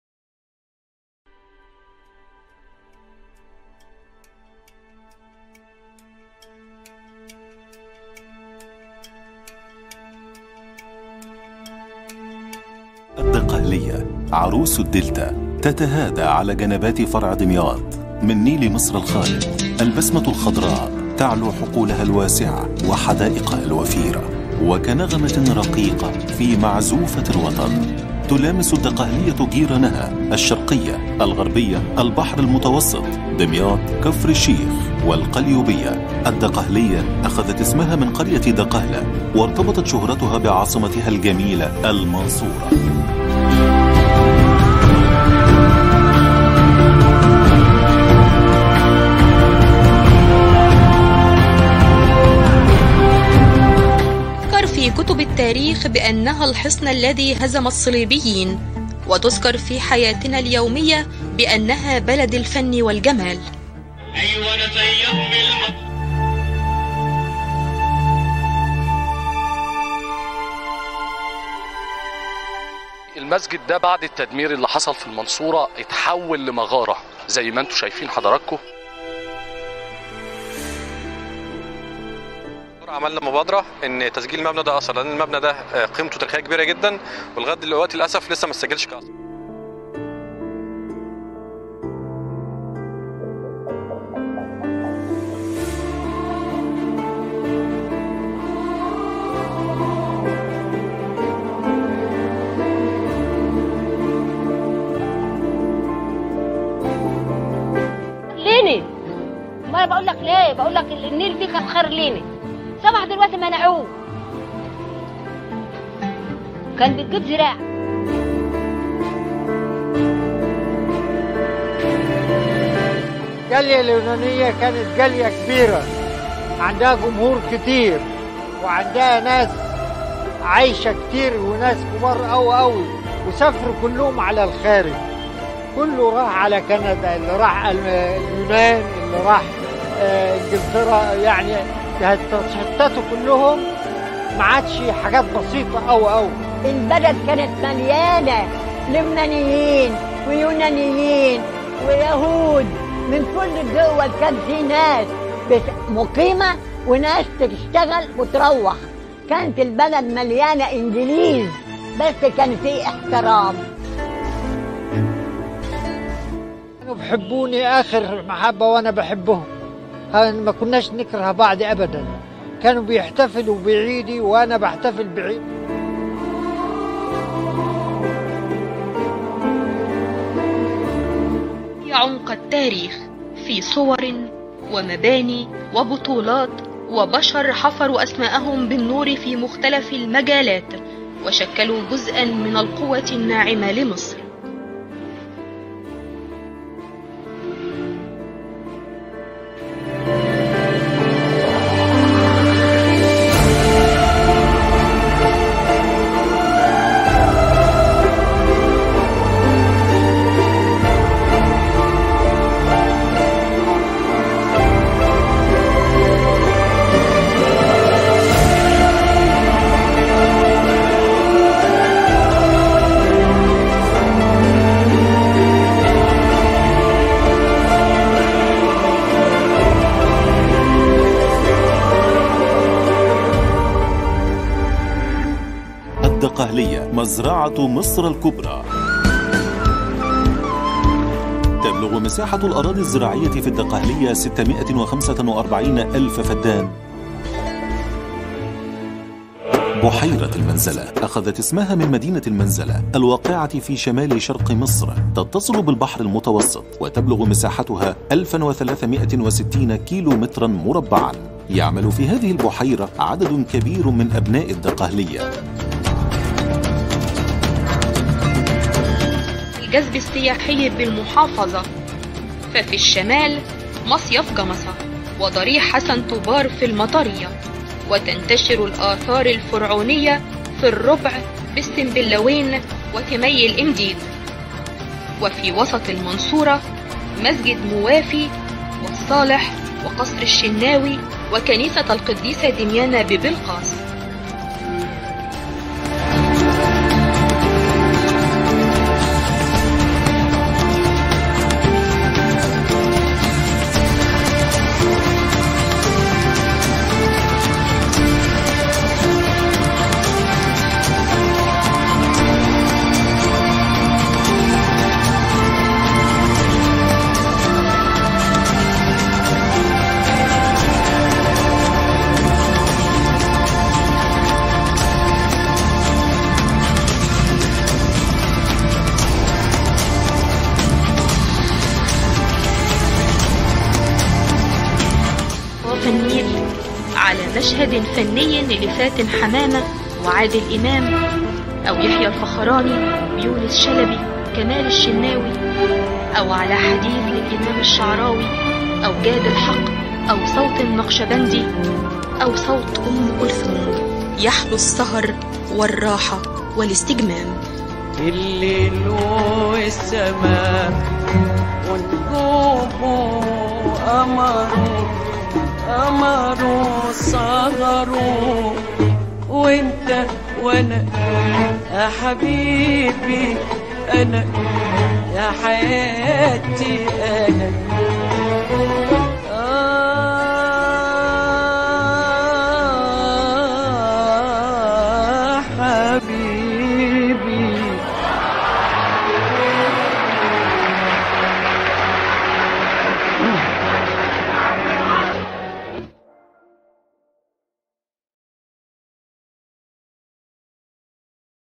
Faculty of Fine Arts - Mansoura University - Research of Graduation Project - 2020/2021
استراتيجية التنمية العمرانية والرؤية المستقبلية والمشروعات الداعمة لإعادة احياء القيمة الفنية والثقافية لمحافظة الدقهلية